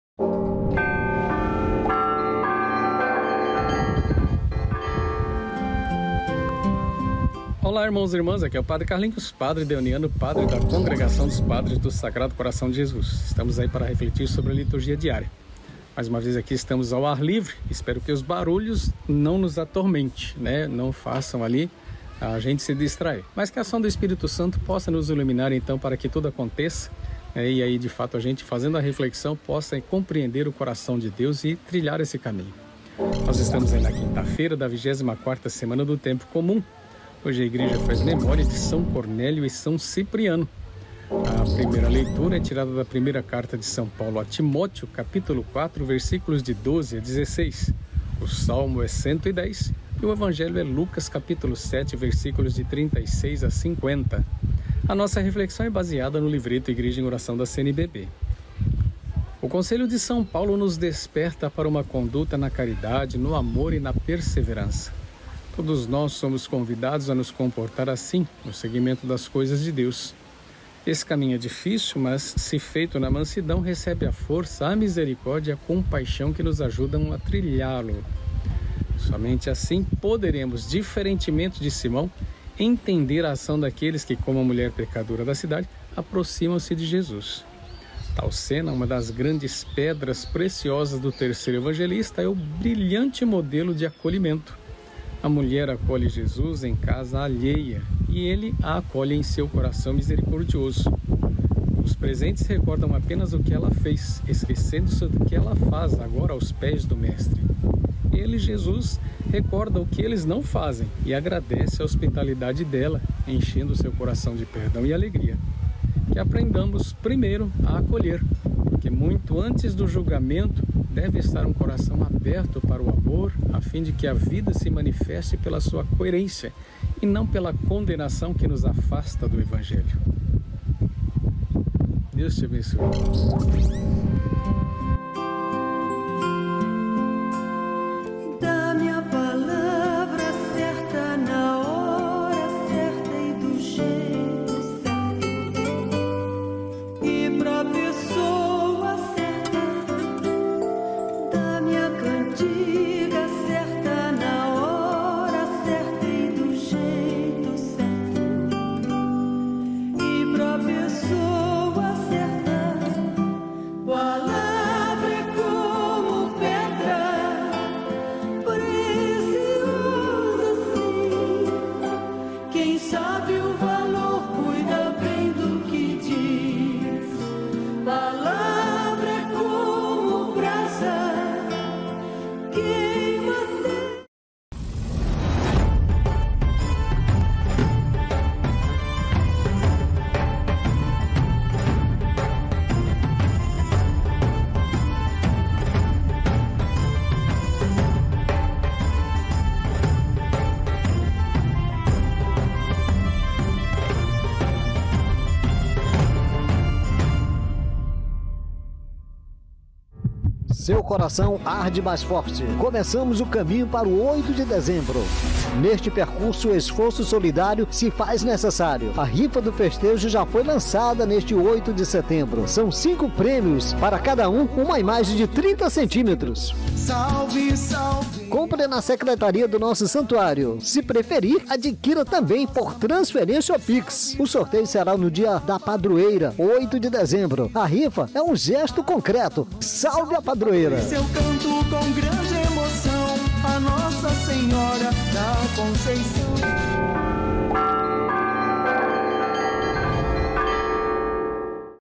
HOMILIA DIÁRIA 16 de Setembro de 2021 - 24ª Semana do Tempo Comum | São Cipriano | Quinta Feira
Acompanhe a homilia diária do nosso Pároco e Reitor do Santuário da Conceição, Padre Carlinhos Paris, scj.